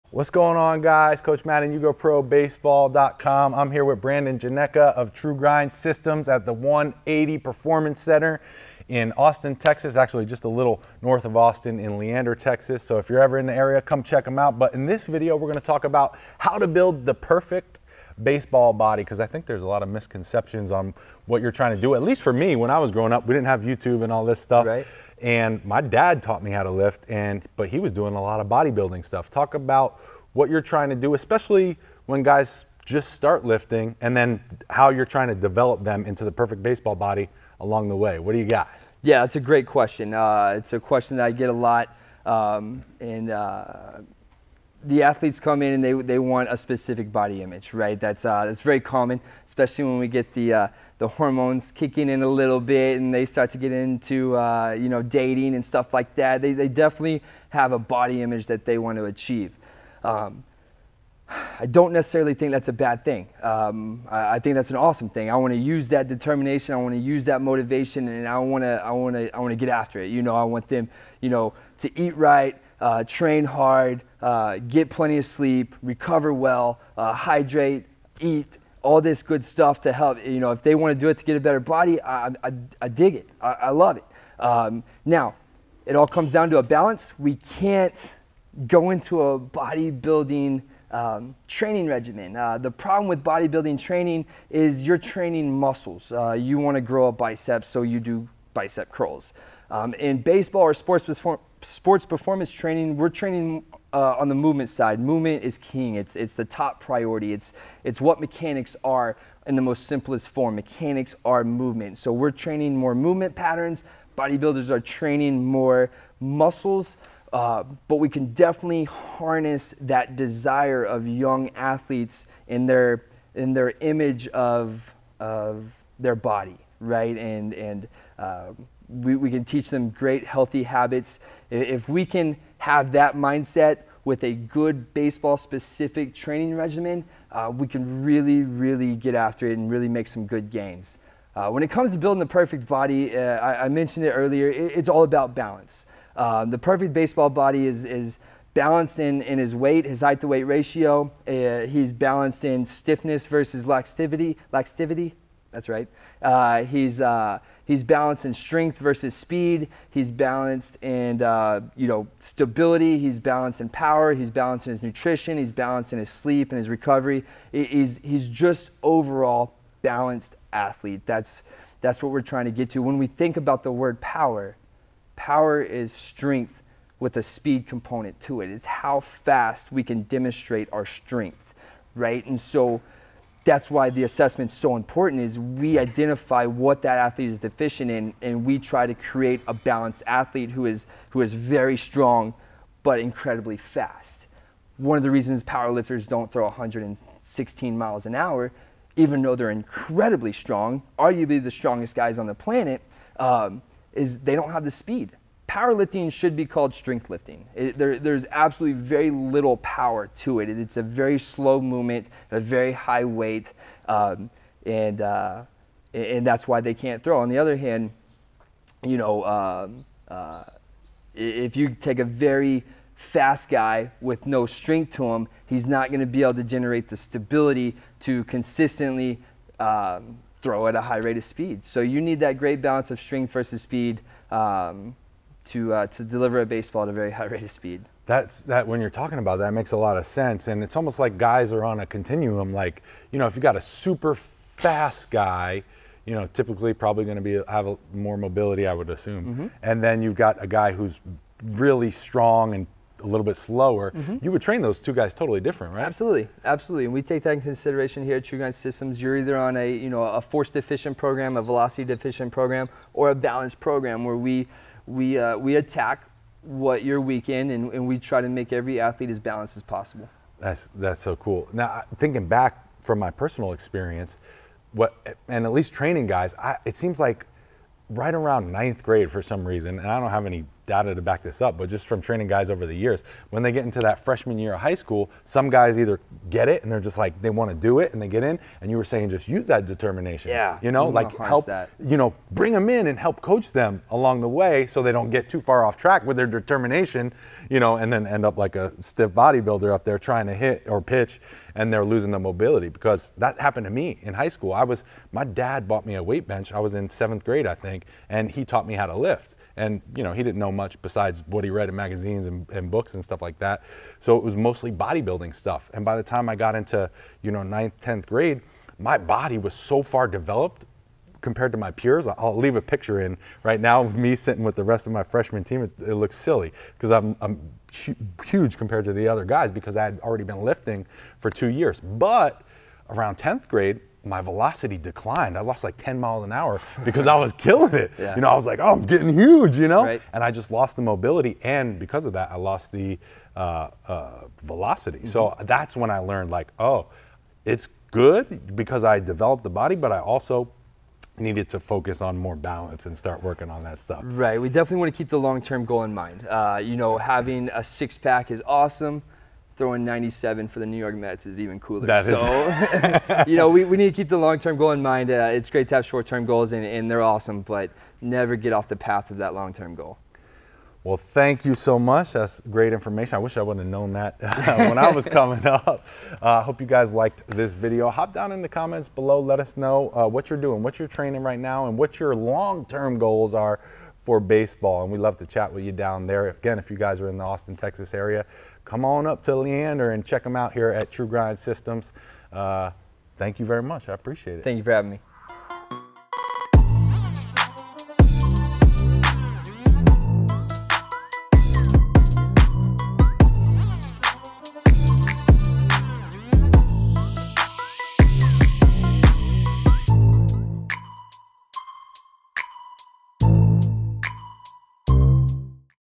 How To Train THE RIGHT WAY For Baseball 💪⚾ (Building The Perfect Baseball Body!)
In this video we're talking with Brandon Janecka of True Grind Systems at 180 Performance Center in Leander Texas (just north of Austin) about how to build the perfect baseball body.  We talk about workout strategies like body building vs power lifting vs sport specific training for baseball.  Give this video a watch if you're serious about taking your players game to the next level.  Here are the details of the video broken down by time stamps:

00:00 Introducing Brandon Janecka
00:39 Working with athletes that have a specific body image in mind
01:25 The problem with body building for baseball players
02:21 The perfect baseball is balanced in many areas
02:52 About Power in Baseball
03:57 The Speed vs Strength Continuum
04:31 Using Determination to train the right way
05:54 Keep the long term goal in mind
06:16 What's next?

📍If you're in the Austin TX area I definitely suggest heading up to Leander to get some training at the 180 Performance Center.

💪Here are some of the best baseball exercise videos from The YouGoProBaseball YouTube Channel:

Video Production & Post Production:  Kevin Chan @Chanterprise

If you've got any questions or comments, hop down below and let's chat there!